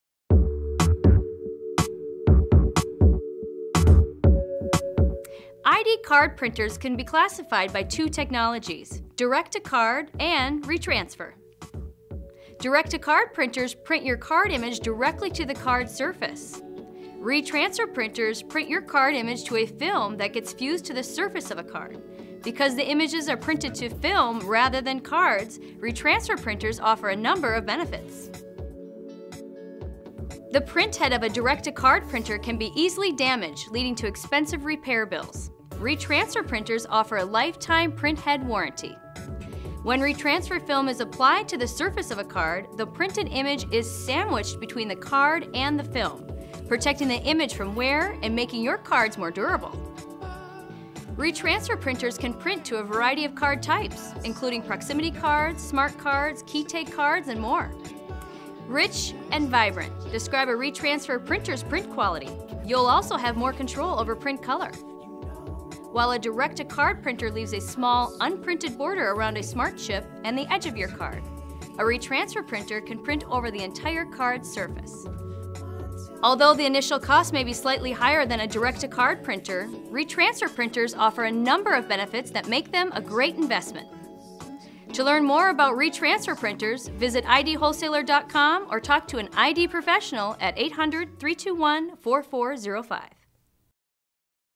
Top 5 Benefits of Retransfer ID Card Printers
- Retransfer printing technology has many benefits for printing ID cards, including added card durability and true over-the-edge printing. Watch this short video to learn about the top five benefits of printing your cards with a retransfer ID card printer.

Read helpful customer reviews while you shop and compare ID card printers